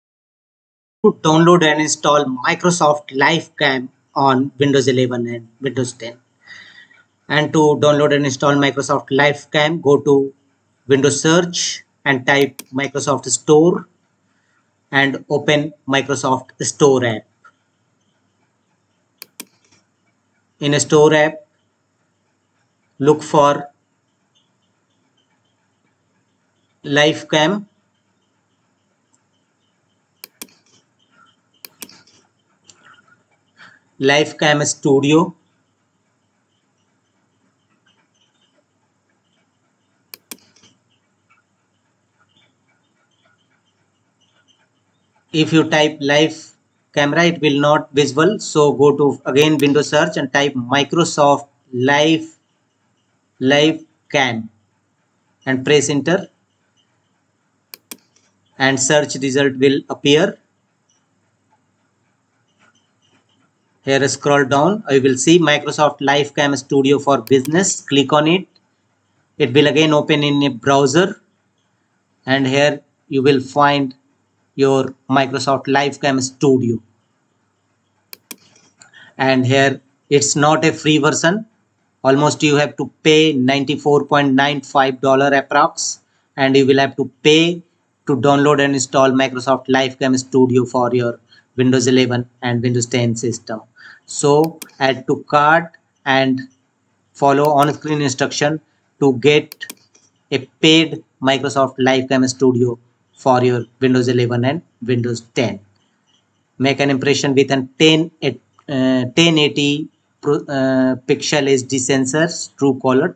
Download and Install Microsoft LifeCam on Windows 11 / 10
How to Download and Install Microsoft LifeCam on Windows 11 / 10 through Microsoft Store.